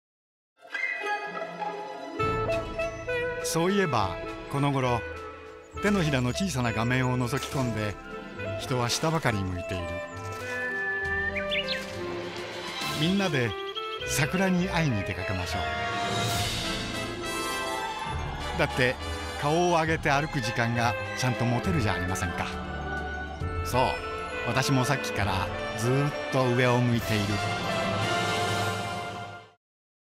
【TVCM】2017年 春「二条城」そうだ 京都、行こう。
そう言えば、この頃、手のひらの小さな画面を覗き込んで、
人は下ばかり向いている。

みんなで、桜に会いに出掛けましょう。

だって、顔を上げて歩く時間が、
ちゃんと持てるじゃありませんか。

そう、私もさっきからずーと上を向いている。

◆編曲者：清水靖晃
◆撮影協力：二条城
※特別な許可を得て撮影している場所及びシーンが含まれています。
※画像はイメージです。

最新の観光情報、おすすめスポット情報はこちら。